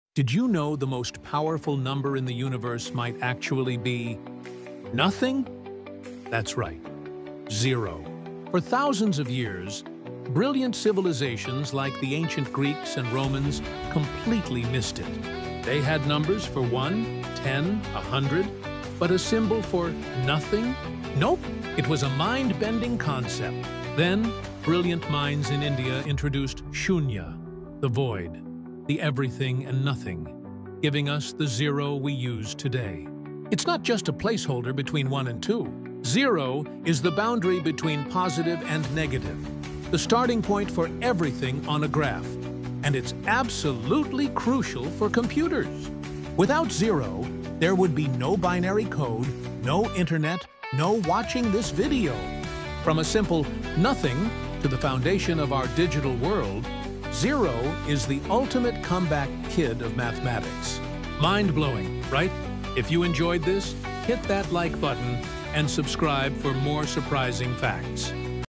🔢 Why Zero Is Both Nothing and Everything | Mind-Blowing Math Facts
Zero — it seems like nothing, but it’s everything! 🌌
In this video, we explore how 0 changed mathematics, science, and our understanding of the universe. From ancient civilizations to modern technology, zero shows us the power of “nothing.”

Discover why zero represents emptiness, infinity, and balance all at once. Perfect for curious minds who love math, logic, and the mysteries of numbers!